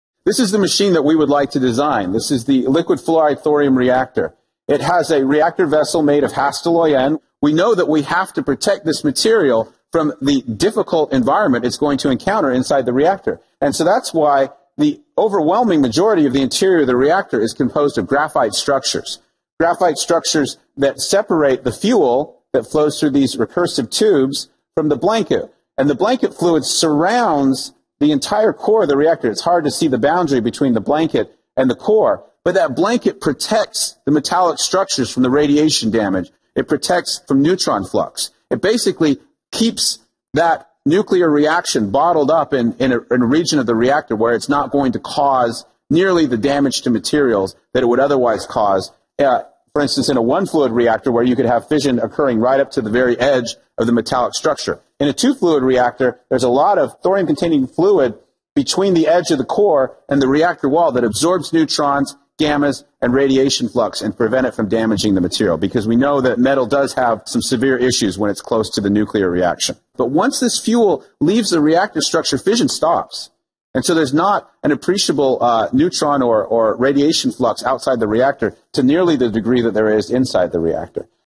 4h05m49s18f LFTR - Hastelloy-N - 2-Fluid FLiBe - Blanket Protects Materials - TR2016a
Liquid Fluoride Thorium Reactor. It has a reactor vessel made of Hastelloy-N. We know that we have to protect this material from the difficult environment it's going to encounter inside the reactor. And so that's why the overwhelming majority of the interior of the reactor is composed of graphite structures. Graphite structures that separate the fuel that flows through these recursive tubes from the blanket. And the blanket fluid surround the entire core the reactor. It's hard to see the boundary between the blanket and the core, but that blanket protects the metallic structures from the radiation damage. It protects from neutron flux. It basically keeps that nuclear reaction bottled up in a region of the reactor where it's not going to cause nearly the damage to material that would otherwise cause. For instance, in a one-fluid reactor where you could have fission occurring right up to the very edge of the metallic structure. In a two-fluid reactor there's a lot of thorium containing fluid between the edge of the core and the reactor wall that absorbs neutrons, gammas, and radiation flux and prevent it from damaging the material. Because we know that metal does have some severe issues when it's close to the nuclear reaction. But once this fuel leaves the reactor structure, fission stops. And so there's not an appreciable neutron or radiation flux outside the reactor to nearly to the degree that there is inside the reactor. -Kirk Sorensen